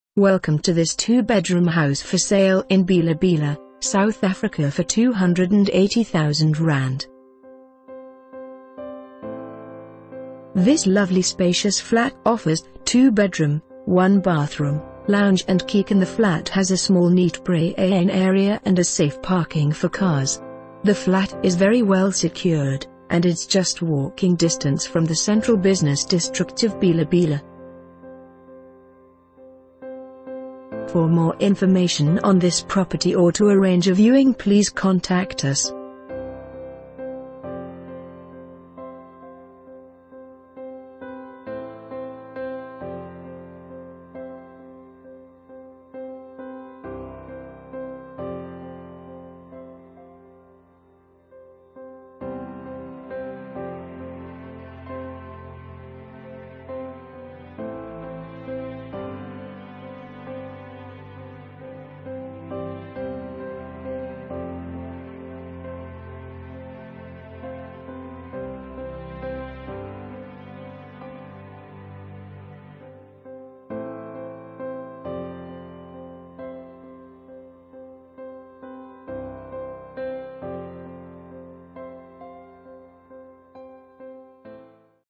2 Bedroom House For Sale in Bela-Bela, South Africa for ZAR 280,000...
This lovely spacious flat offers 2 bedroom, 1 bathroom, lounge and kichen. The flat has a small neat  braaing area and a safe parking for cars. The flat is very well secured, and it's just walking distance from the central business district of Bela-Bela.